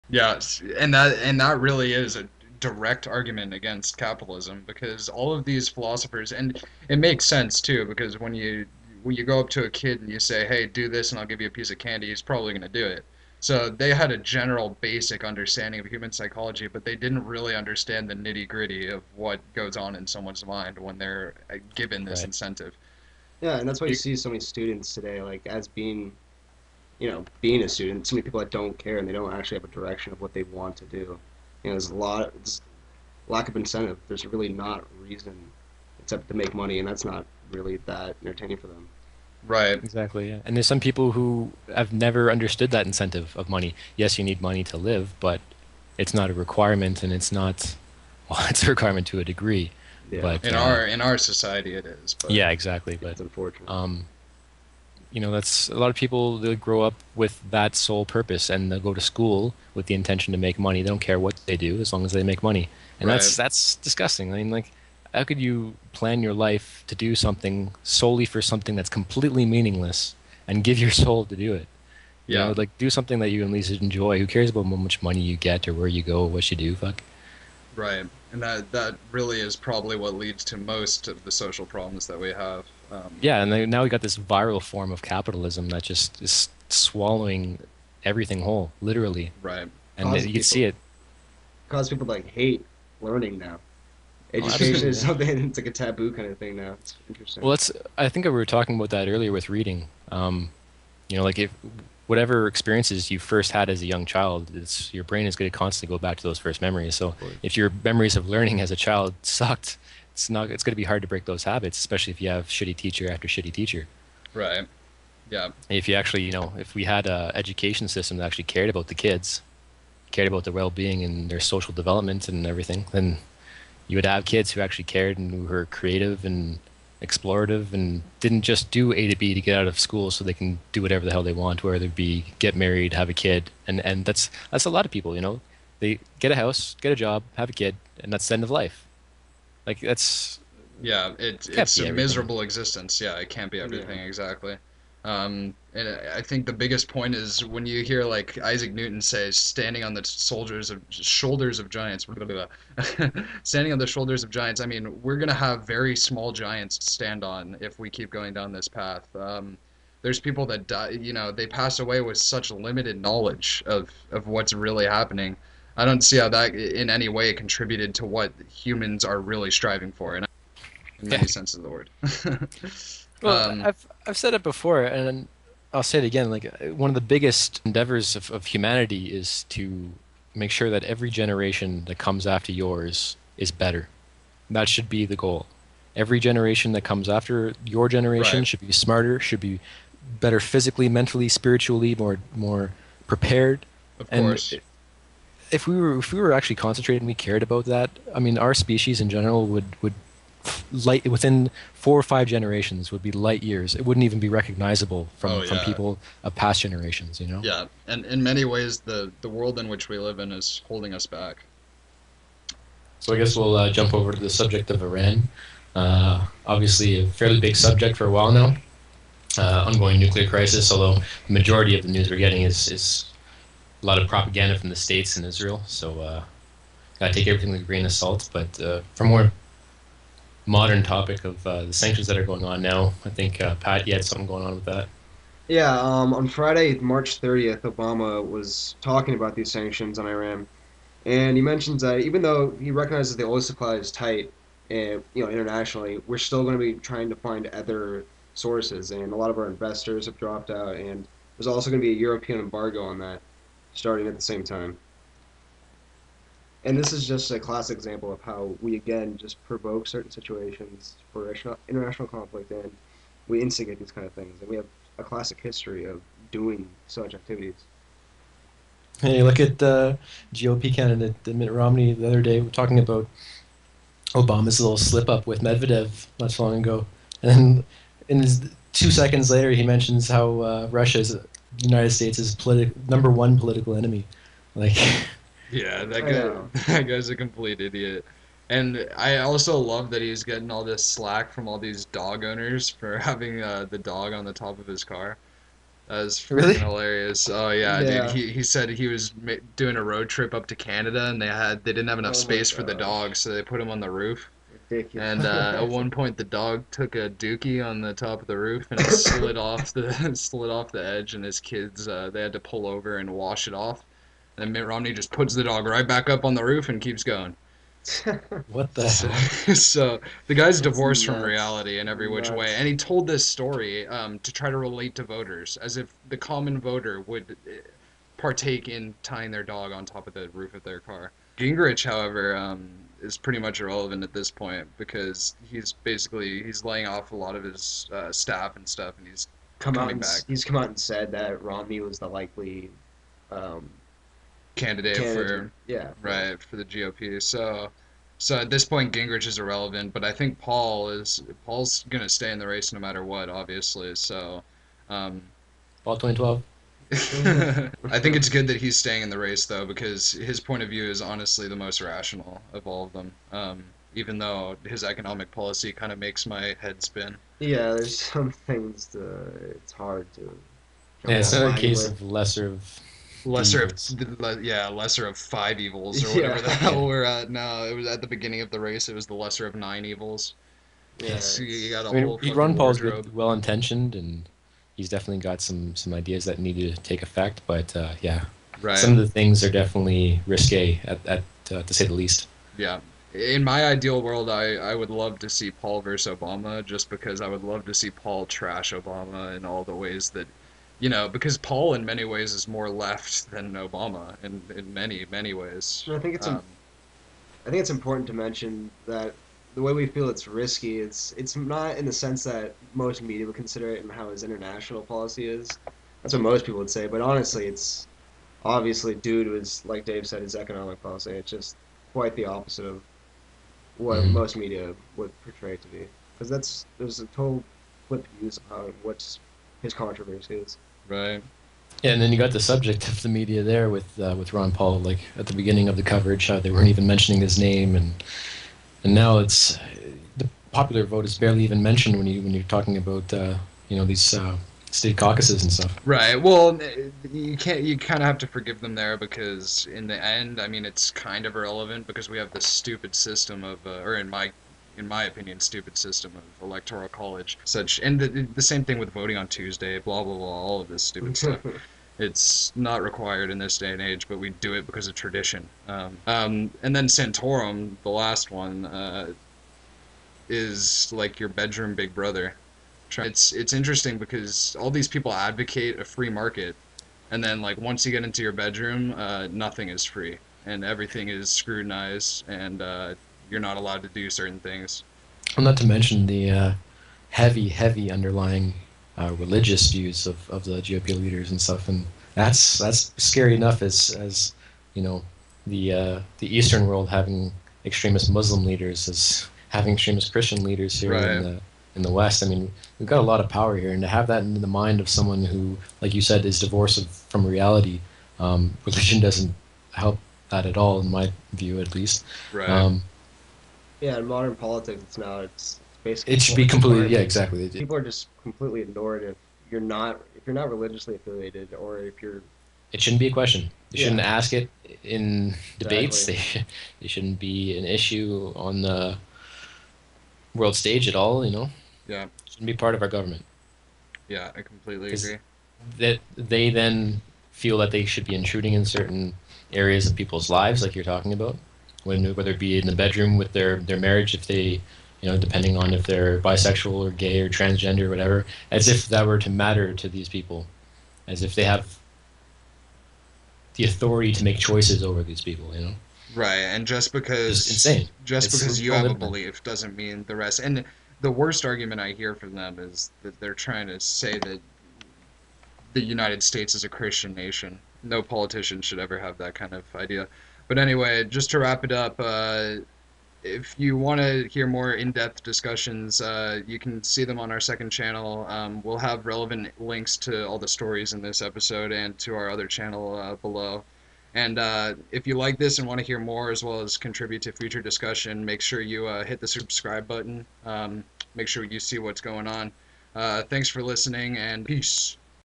Unnamed Podcast March 25-April 1 2012 (3/3)
(Third Part of 3) News show for March 25-April 1st by Mike, Dave, and Pat. Please tell us what you think abou the topics; this is very much a community webshow. *All images from Google.com* *Music by Ronald Jenkees; used with permission*